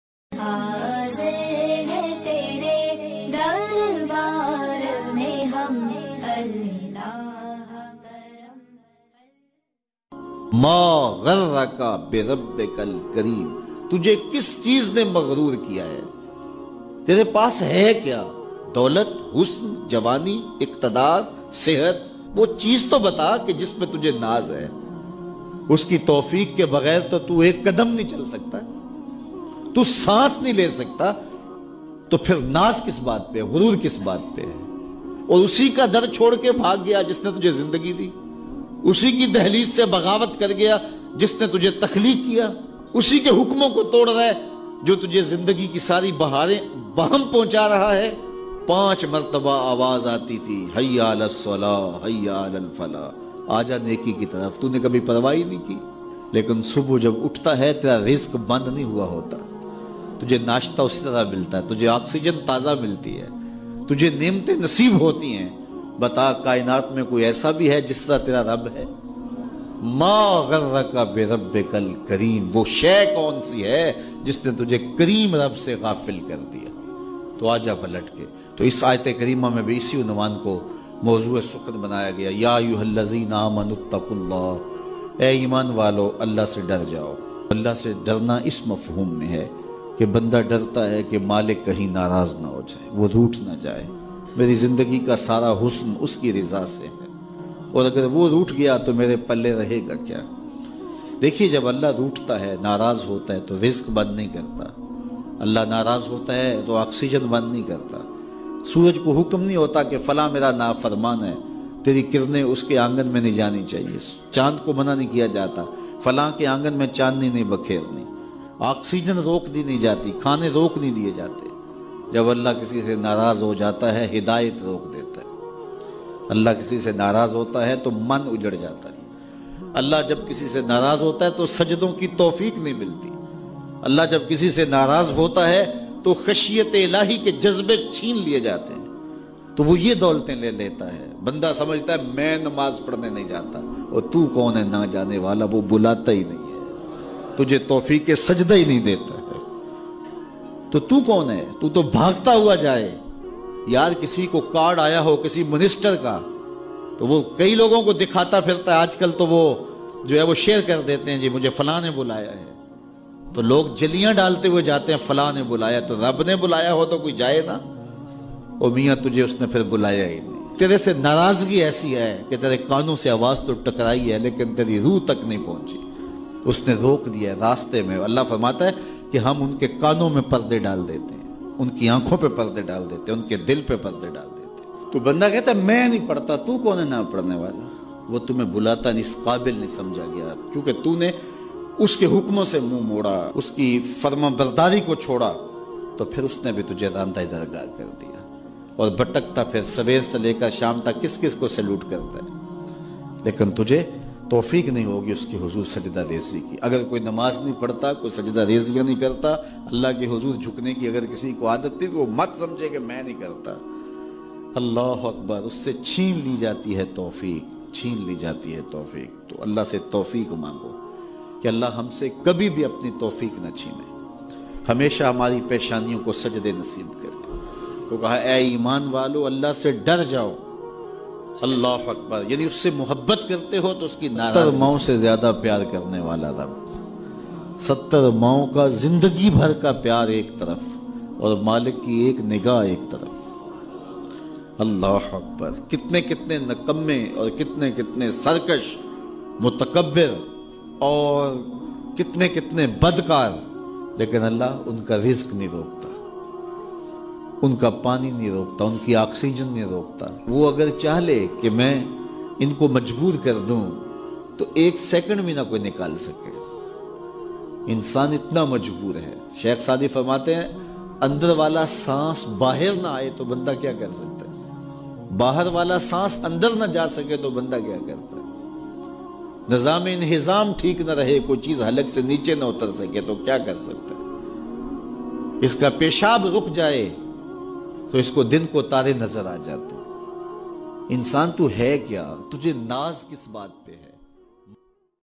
Bayan Of Allama Saqib raza mustafai Sahab (کہ انسان تجھے کس بات کا غرور ہے تو ہے کیا) |ZM Studio
Bayan Of Allama Saqib raza mustafai Sahab🎤
 کہ انسان تجھے کس بات کا غرور ہے تو ہے کیا